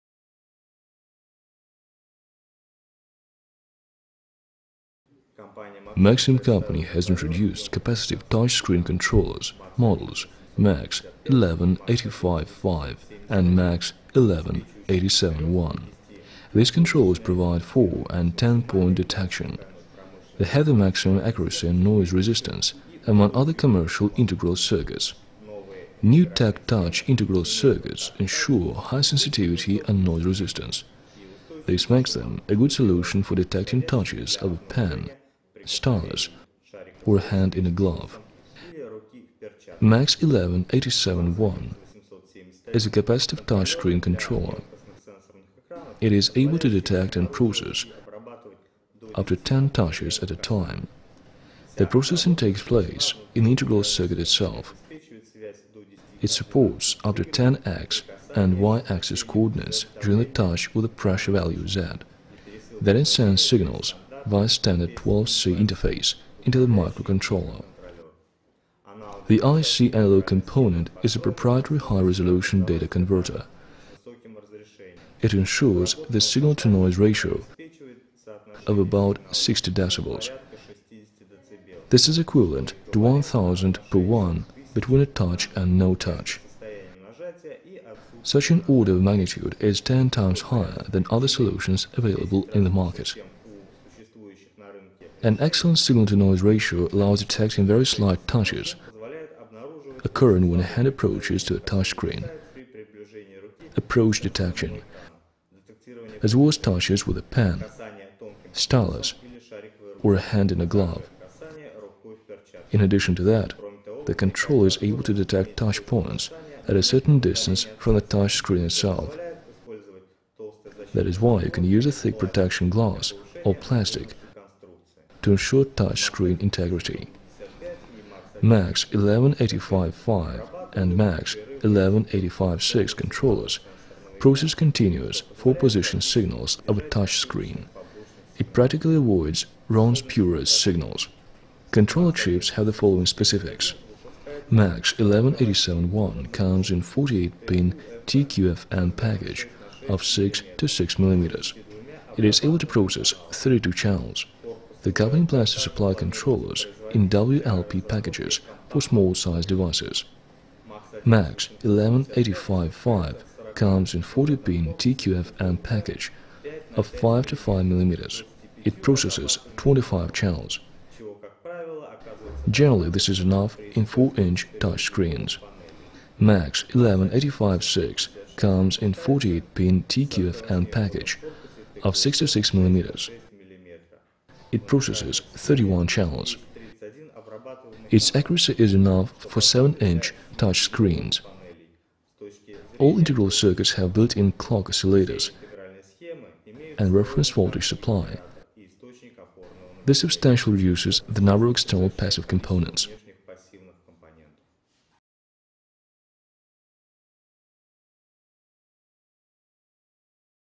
MAX11855 and MAX11871 -- 4 and 10-Touch Screen ...
MAX11855 and MAX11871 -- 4 and 10-Touch Screen Controllers Maxim Company introduced capacitive touch screen controllers, models MAX11855 and MAX11871. These controllers provide 4-and 10-point detection. They have the maximum accuracy and noise resistance among other commercial integral circuits.New TacTouch integral circuits ensure high sensitivity and noise resistance. This makes them a good solution for detecting touches of a pen, stylus or a hand in a glove.MAX11871 is a capacitive touch screen controller. It is able to detect and process up to 10 touches at a time. The processing takes place in the integral circuit itself. It supports up to 10 X and Y-axis coordinates during the touch with the pressure value Z. Then it sends signals via standard I2C interface to the micro controller.The IC analogue component is a proprietary high-resolution data converter. It ensures the signal-to-noise ratio of about 60 dB. This is equivalent to 1000:1 between a touch and no touch.Such an order of magnitude is 10 times higher than other solutions available in the market.An excellent signal-to-noise ratio allows detecting very slight touches occurring when a hand approaches to the touch screen (approach detection) as well as touches with a pen, stylus or a hand in a glove. In addition to that, the controller is able to detect touch points at a certain distance from the touch screen itself. That's why you can use a thick protection glass or plastic to ensure touch screen integrity.MAX11855 and MAX11856 controllers process continuous 4-position signals of a touch screen. It practically avoids wrong spurious signals.Controller chips have the following specifics:• MAX11871 comes in 48-pin TQFN package of 6mm x 6mm. It is able to process 32 channels. The company plans to supply controllers in WLP packages for small-size devices.• MAX11855 comes in 40-pin TQFN package of 5x5mm. It processes 25 channels. Generally, this is enough for 4-inch touch screens.• MAX11856 comes in 48-pin TQFN package of 6mm x 6mm. It processes 31 channels. Its accuracy is enough for 7-inch touch screens.• All integral circuits have built-in clock oscillators and reference voltage supply. This substantially reduces the number of external passive components